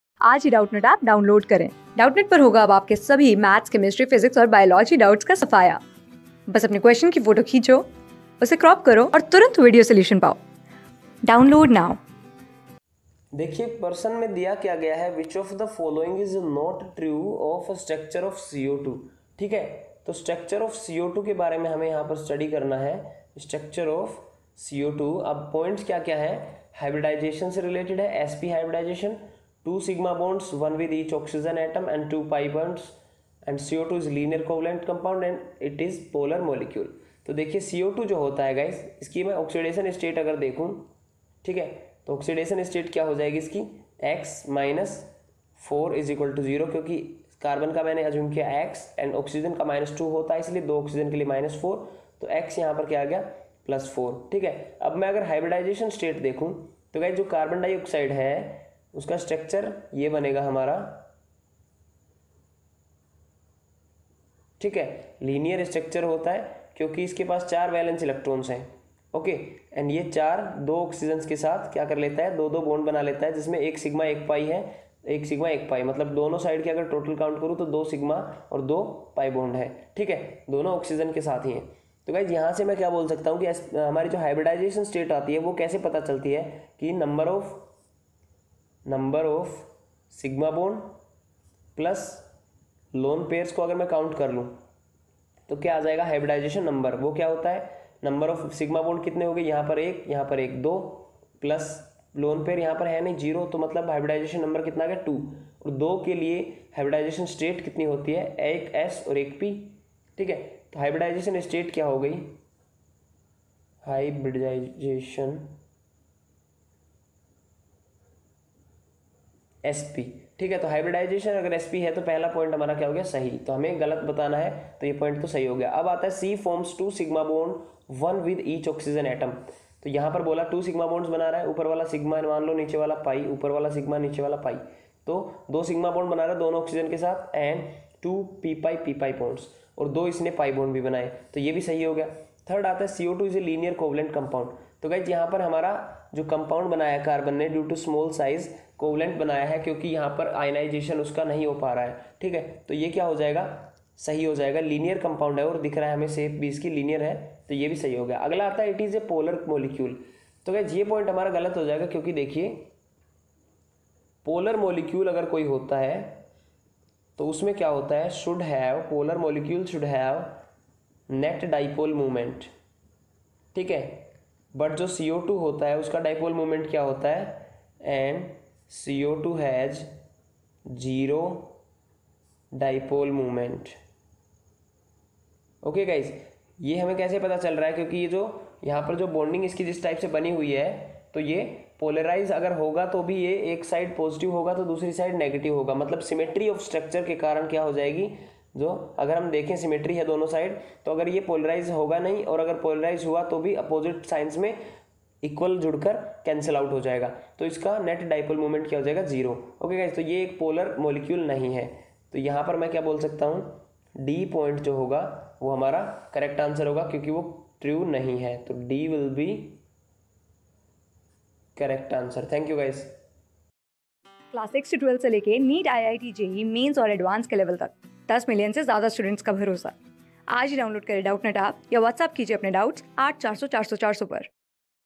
which of the following is not true about structure of carbon dioxide? | CLASS 11 | THE P-BLOCK E...
which of the following is not true about structure of carbon dioxide?
Class: 11
Subject: CHEMISTRY
Chapter: THE P-BLOCK ELEMENTS
Board:NEET

👉 Have Any Query? Ask Us.